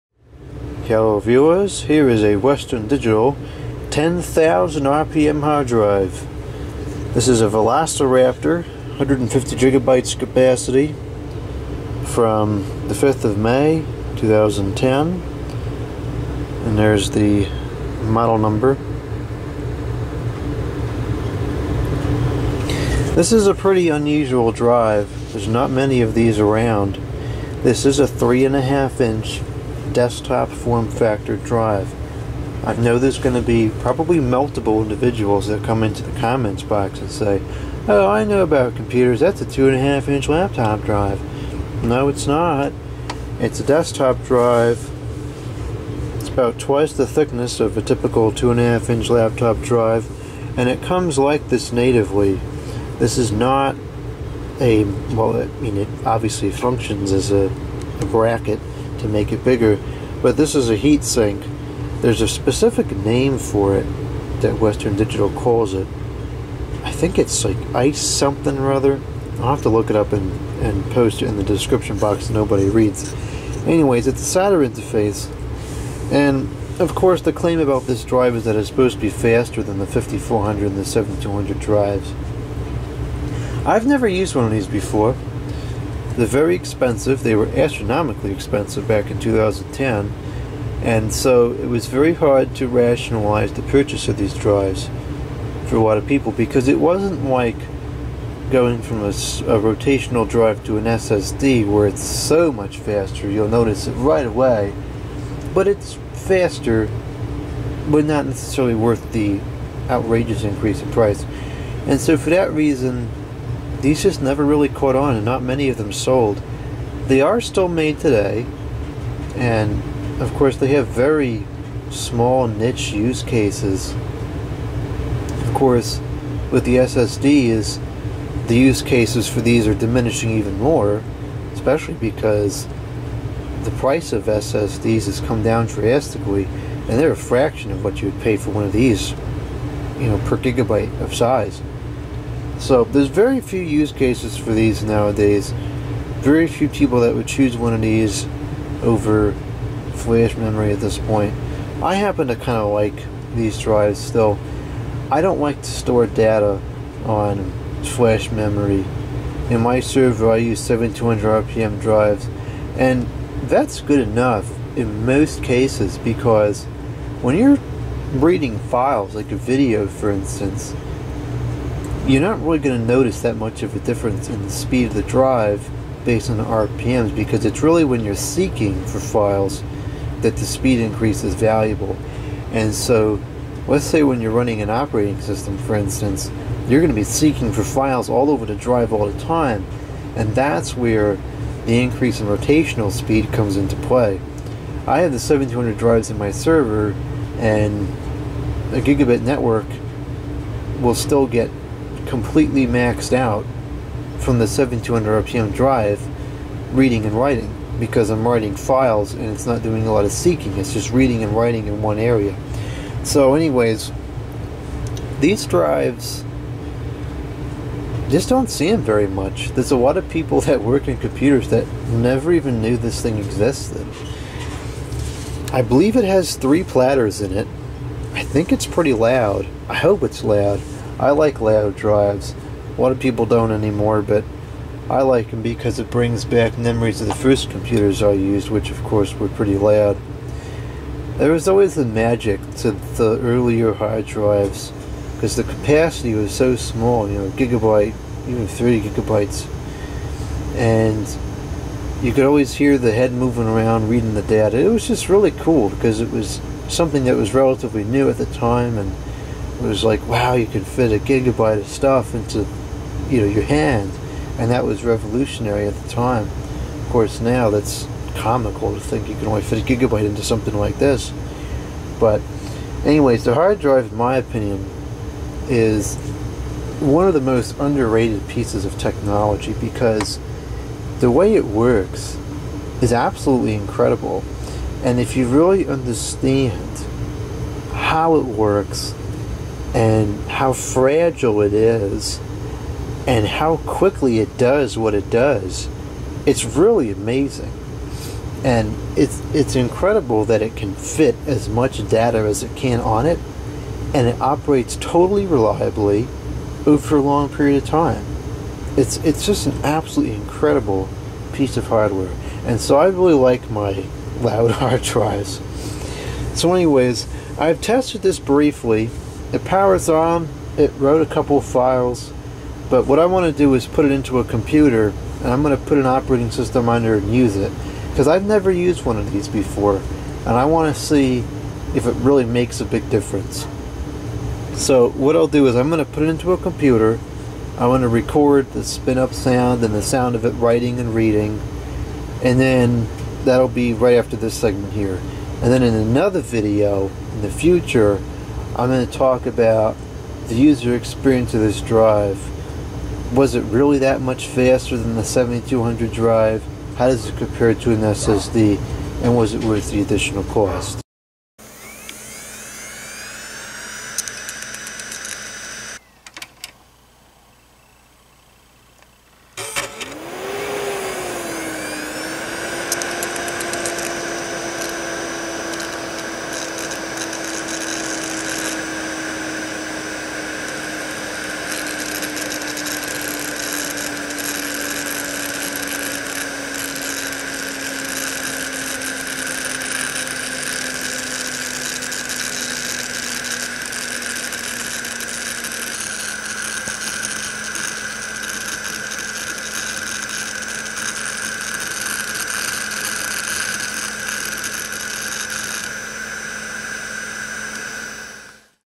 Western Digital WD1500HLFS VelociRaptor 10,000 RPM Hard Drive | Initial Checkout
A rather uncommon Western Digital VelociRaptor 10,000 RPM hard drive, model WD1500HLFS.
The black cooler the drive is mounted in is referred to as an 'Ice Pack'.